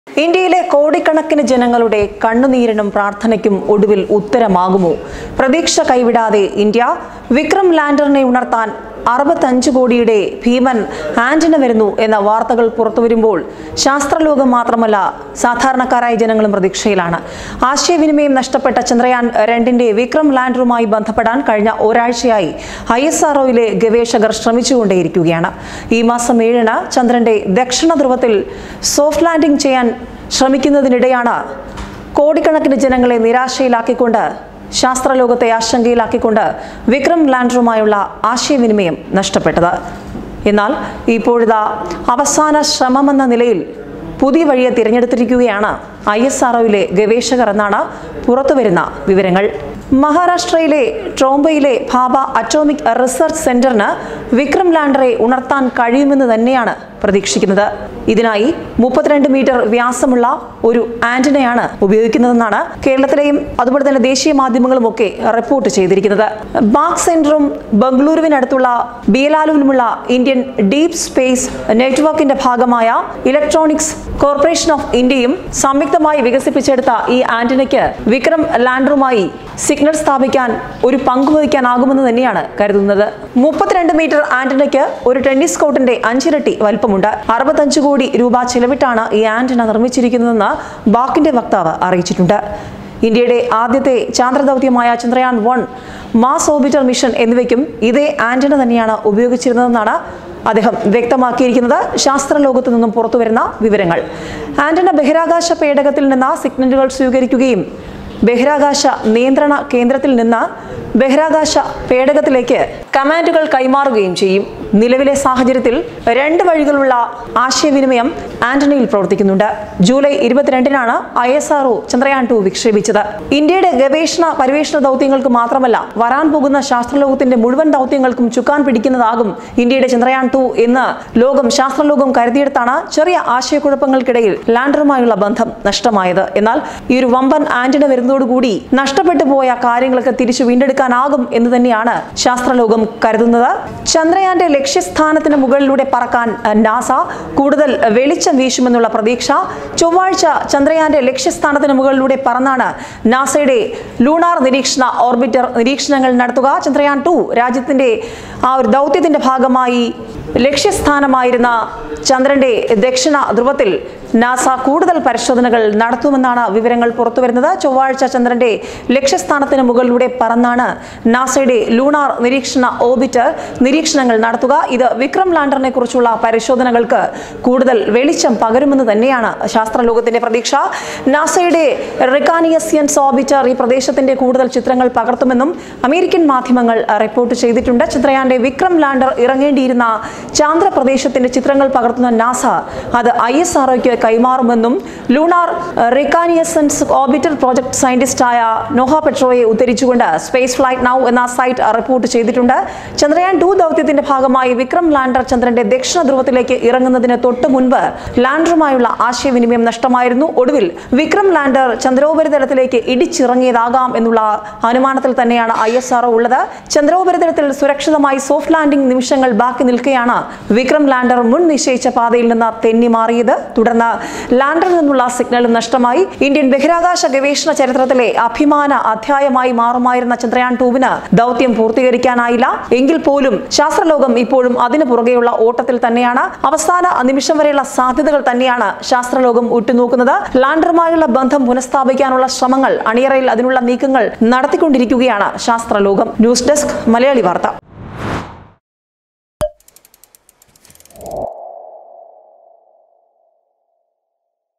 Science ലാന്‍ഡറിനെ ഉണര്‍ത്താന്‍ ഭീമന്‍ ആന്റിന, ആ കണ്ണീരിനും പാര്‍ഥനയ്ക്ക് ഉത്തരം
ലാന്‍ഡറിനെ ഉണര്‍ത്താന്‍ ഭീമന്‍ ആന്റിന, ആ കണ്ണീരിനും പാര്‍ഥനയ്ക്ക് ഉത്തരം